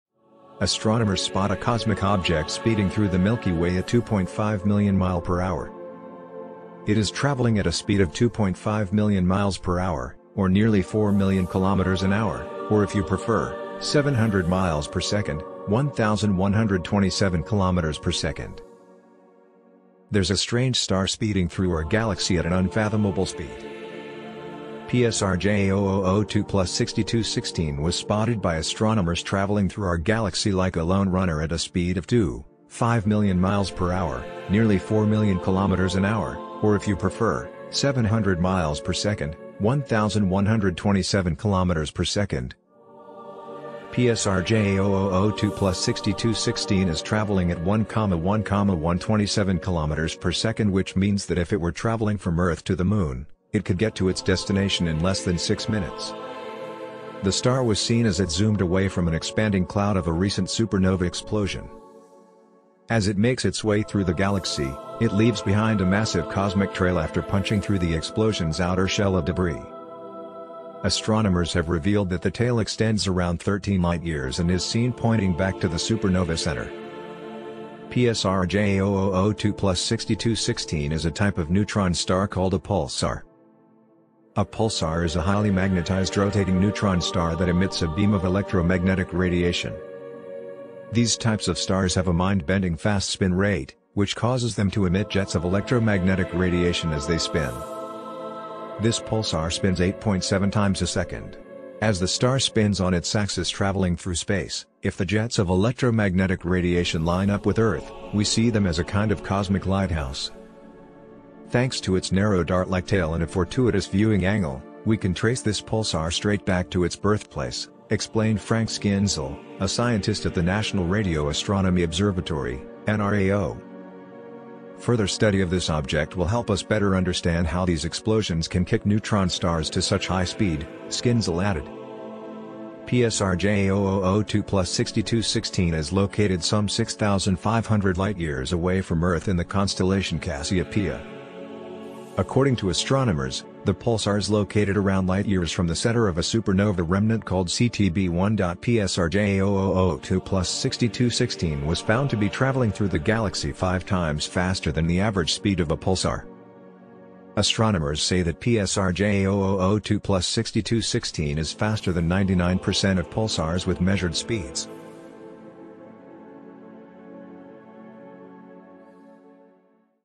Astronomers Spot a Cosmic Object Speeding Through the Milky Way at 2.5 Million Mph
It is traveling at a speed of 2.5 million miles per hour, or nearly four million kilometers an hour, or if you prefer,  700 miles per second (1,127 km/sec).